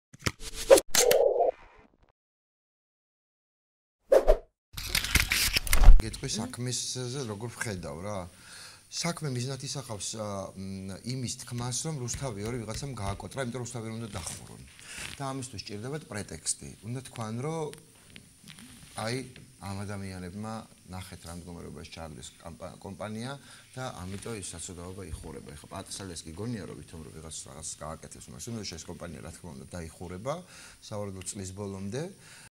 ნიკა გვარამია თვლის, რომ რუსთავი 2-ის მაყურებლის 80 % არის მწვავედ ოპოზიციური
გვარამია ასევე ამტკიცებს რომ სწორედ ამ მიზეზით რუსთავი 2-ის სარედაქციო პოლიტიკის შეცვლა მაყურებლის დაკარგვას გამოიწვევს